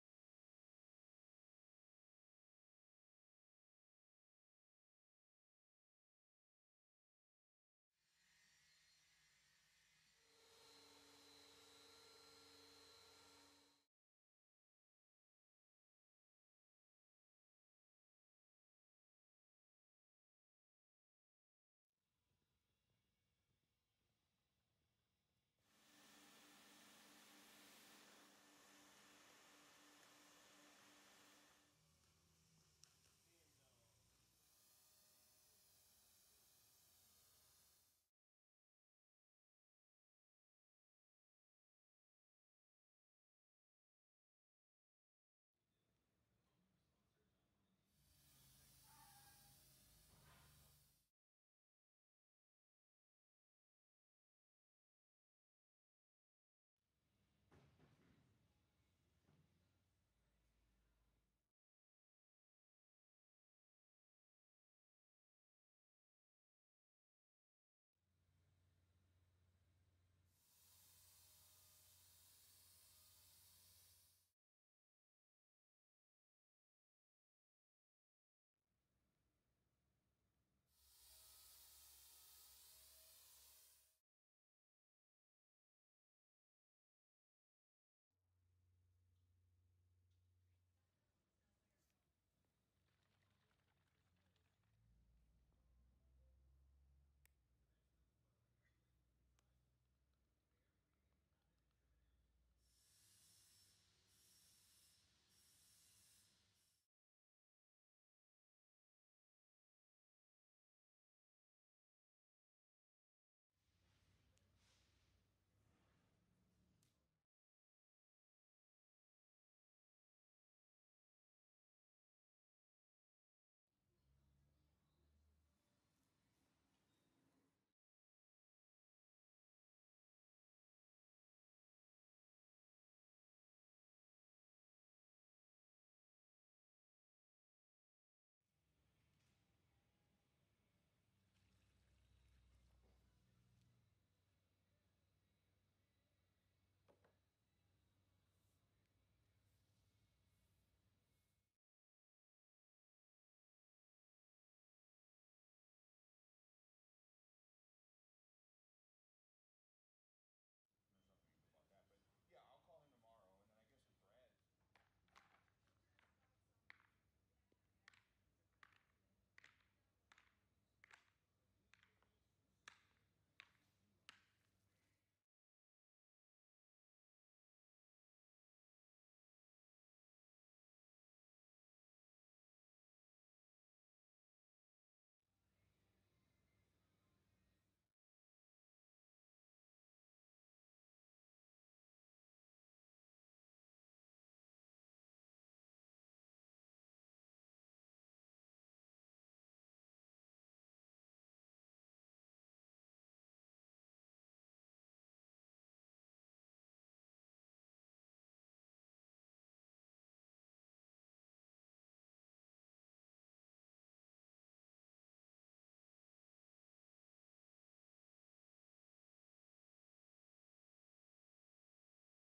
How To: Decorative Concrete Tile Pattern & Seal Coat
Installing a finish such as this requires a plan for the final result and practice working with each of the materials. When the process is completed correctly, the result will be an attractive and durable floor.